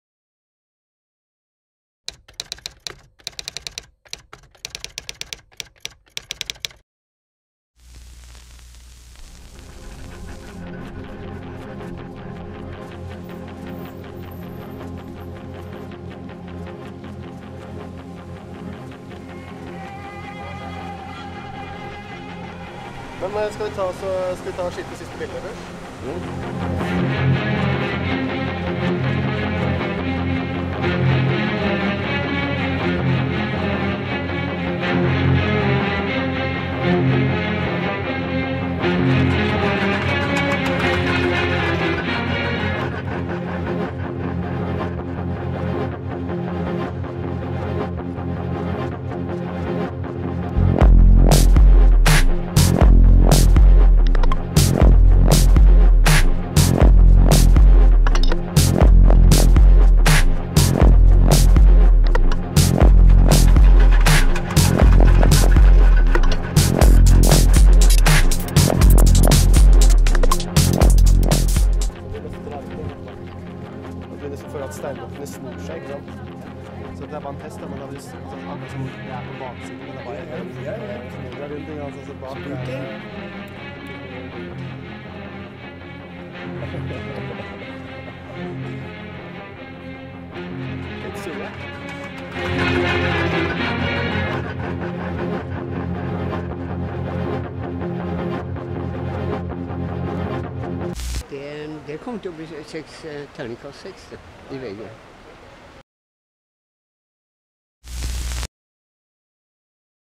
Siste opptaksdag for «Pushwagner»
PUSHWAGNER now available at iTunes!

Bli med bak kulissene til siste opptaksdag for filmen vår om Pushwagner. Even Benestad og August Baugstø Hanssens dokumentar om Pushwagner hadde premiere over hele landet 2. september 2011!

Hariton Pushwagner er en av våre mest markante kunstnere. Bak suksessen ligger en dramatisk fortelling om en vestkantgutt som ble uteligger, for så å ende som en feiret kunstner. Pushwagner selv sier «Det er ikke sant, men det er en god historie». Mot denne bakgrunnen vil filmen reflektere forestillingen om hva som er kunstneren og mennesket Pushwagners virkelighet.